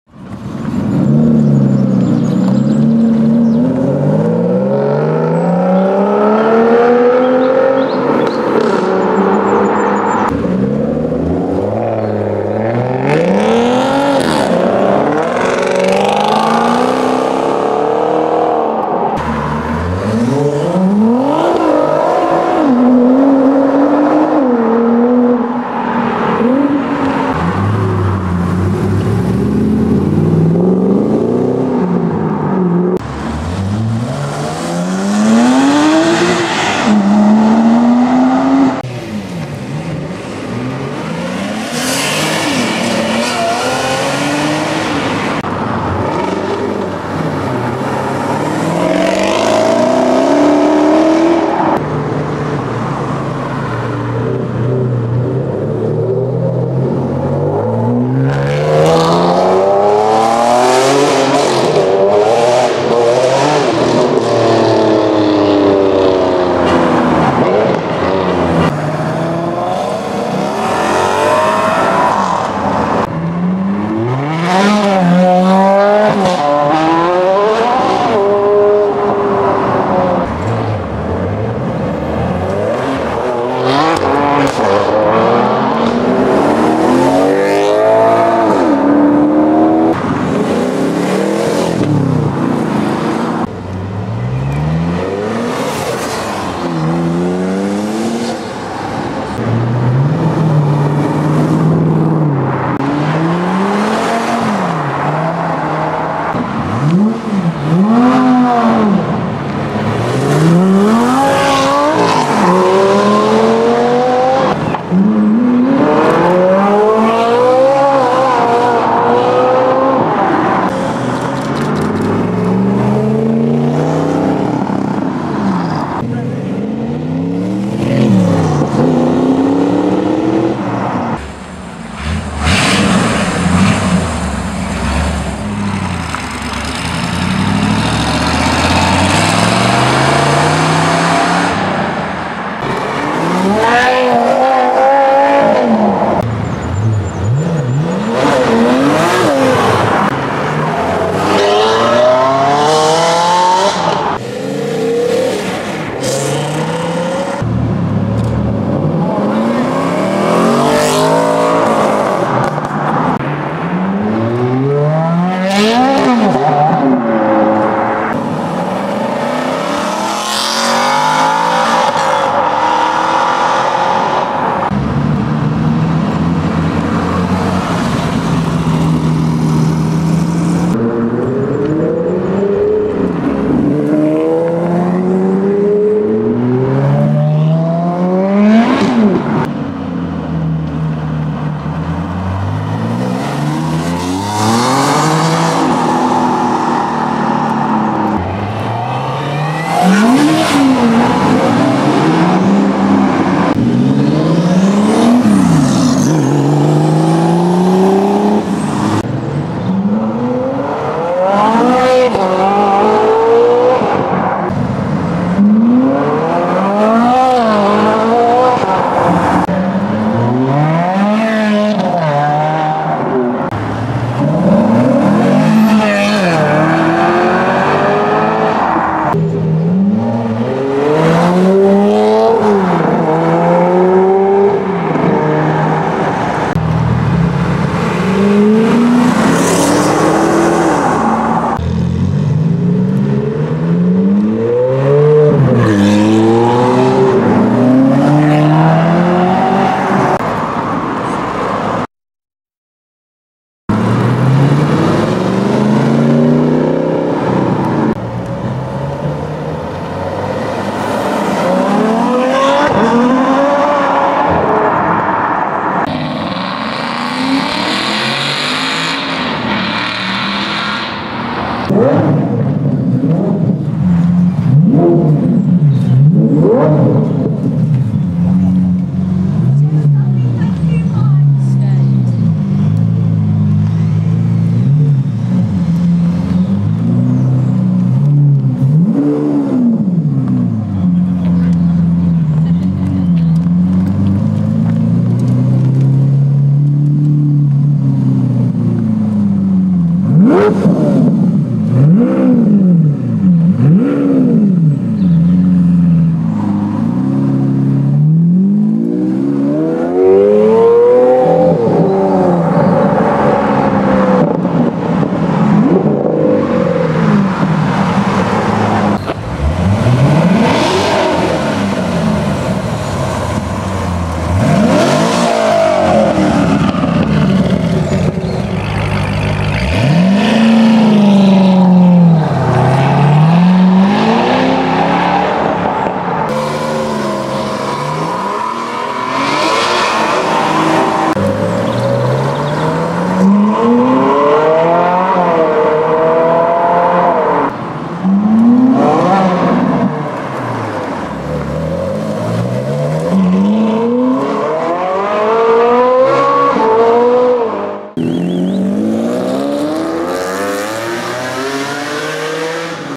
Supercars leaving Wilton Wake-Up breakfast meet (Enzo, F12, Aventador, M600, SV etc)
Wilton House (famous for its summer supercar show) hosts several breakfast meetings for owners (and spectators). I attended the 'supercar' breakfast meet, exclusive to supercars, and recorded them leaving the house in style.